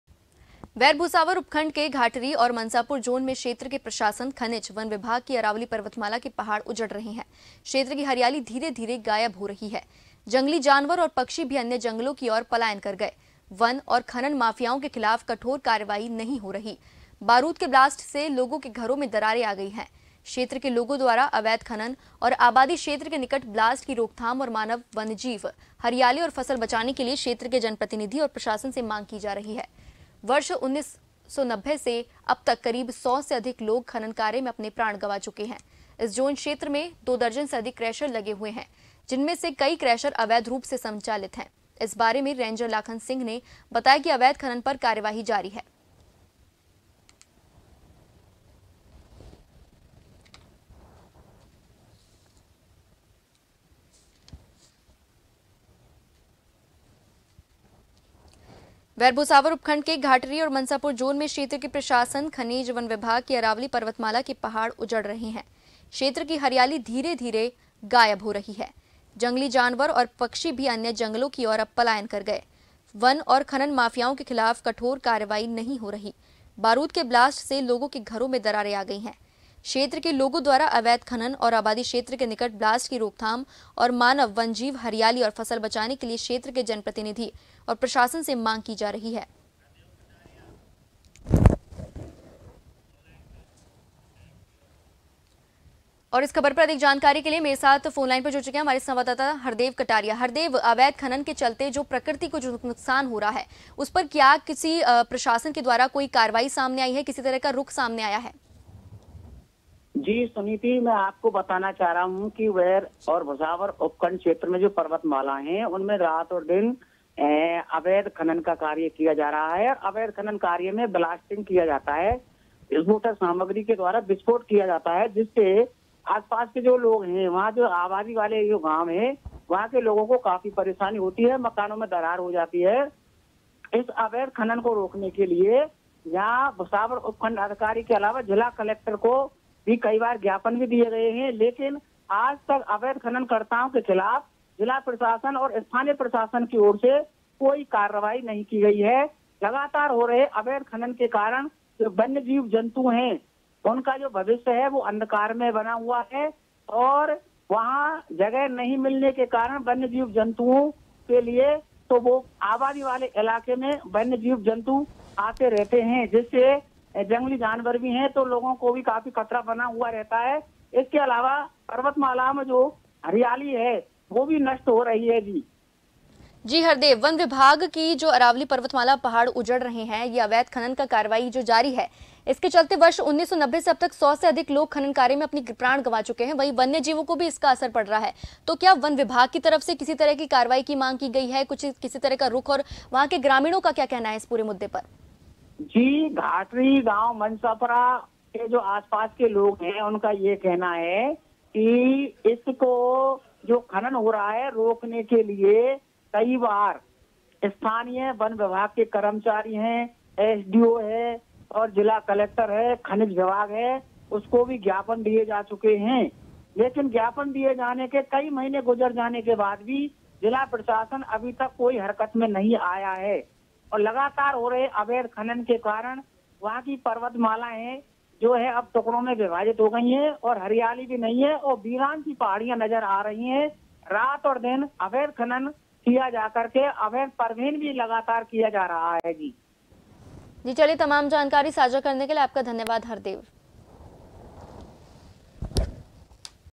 Vair (Raj.) News | घाटरी जोन में उजड़ते पहाड़, गायब होती हरियाली, अवैध खनन का चल रहा काम, प्रशासन मौन
News Credit-MKP